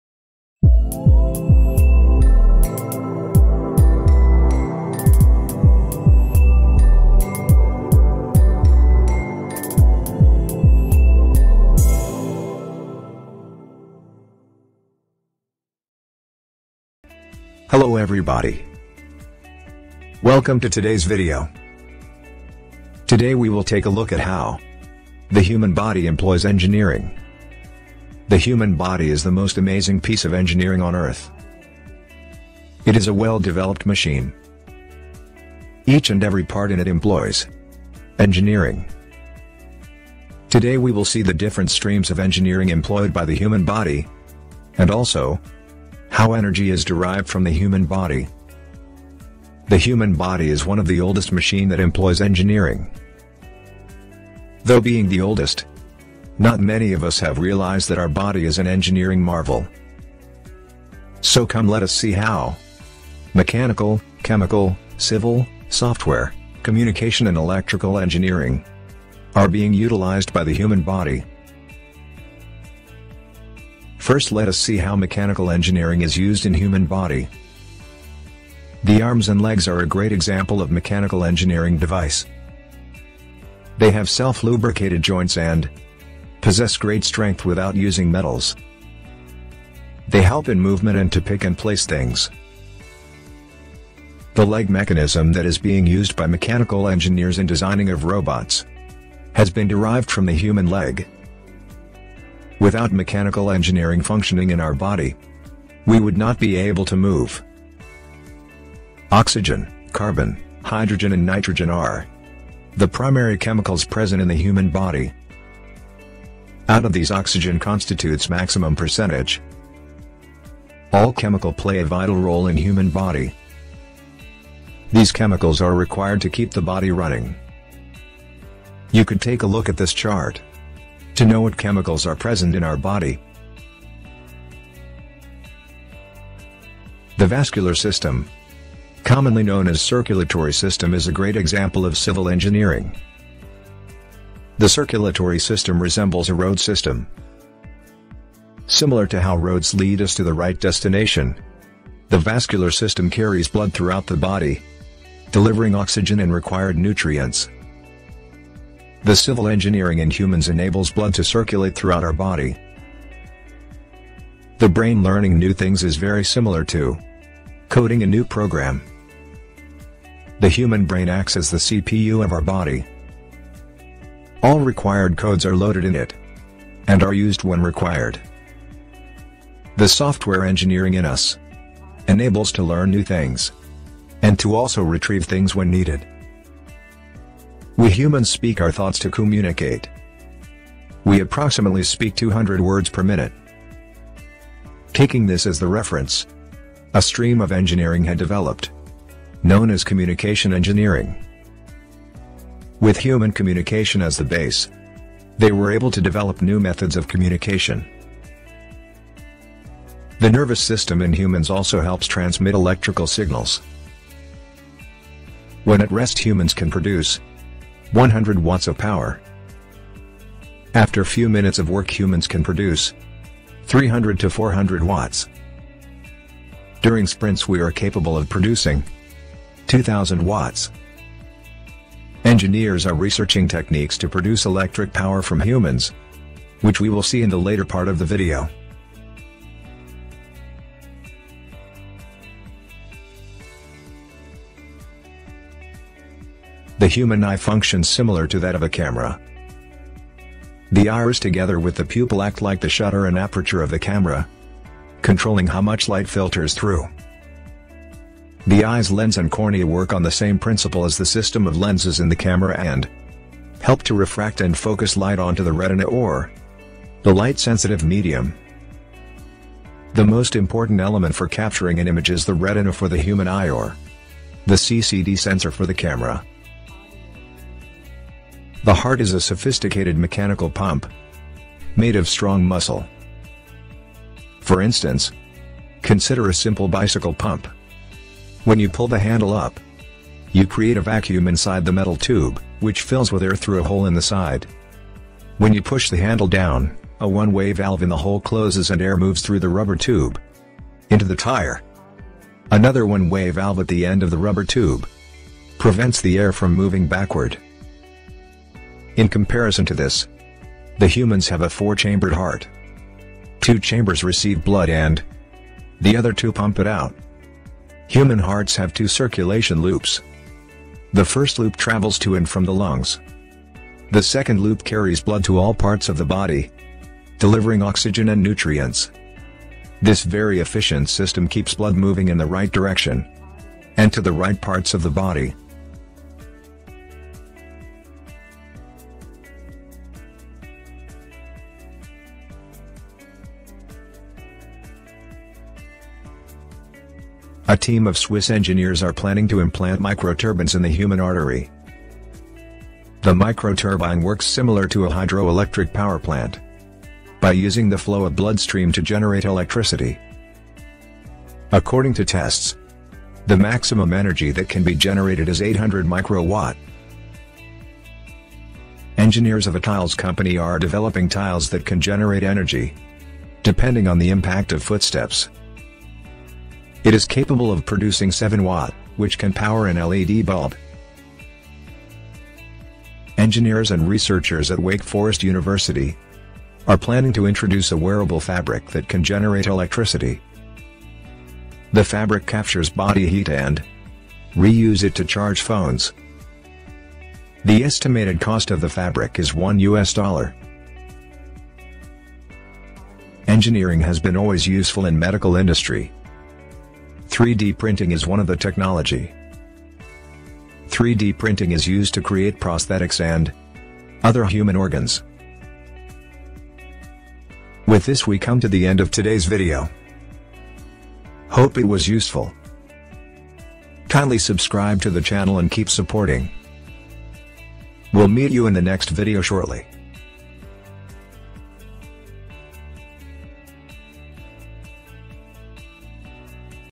Human body and Engineering. The oldest Engineering Marvel.
The video describes how each streams of engineering is being used in the human body. You will get an insight into how the human body is an engineering marvel and also learn about few new technologies being used on human body to generate electricity. These information is given in a simple and elegant way.